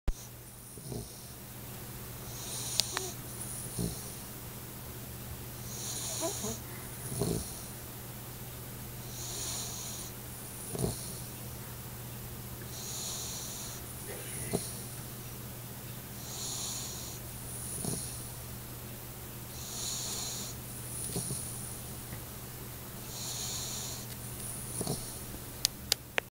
Tuvaaq the Malamute snoring
My 8 week old Alaskan Malamute snoring under my chair.